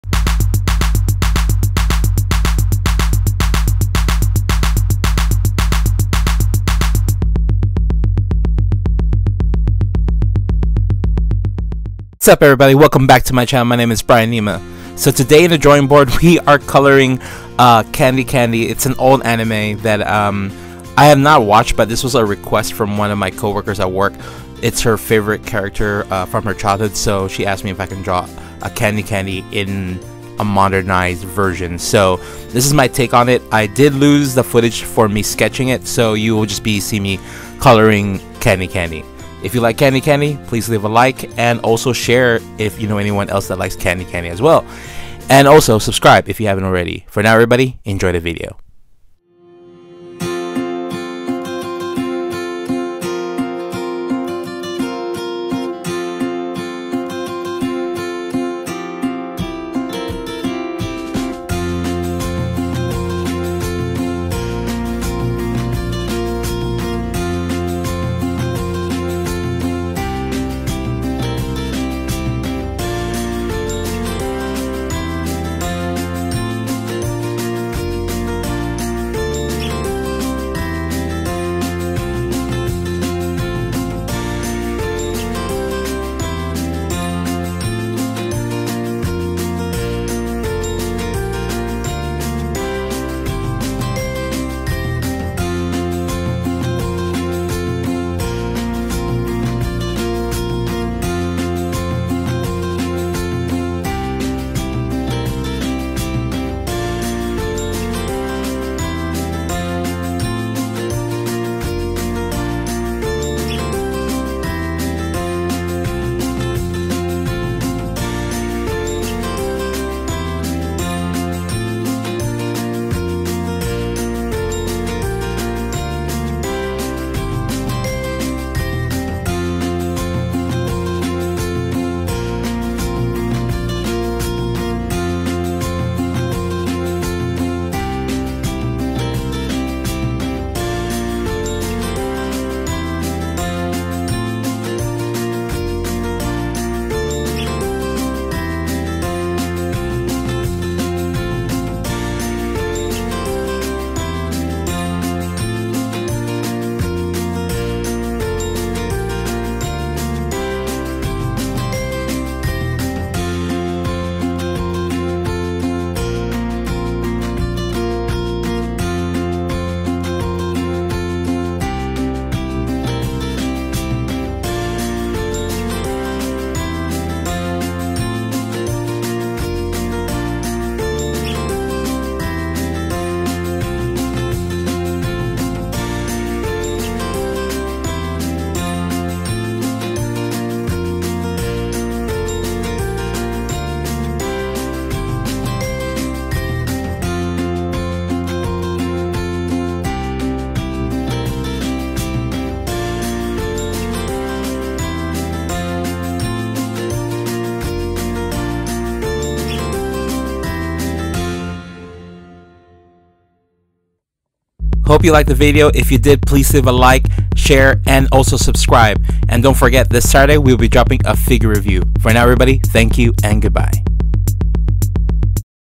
Lets draw Candy Candy
Doing a classic anime a childhood dream to be something sweet Candy Candy.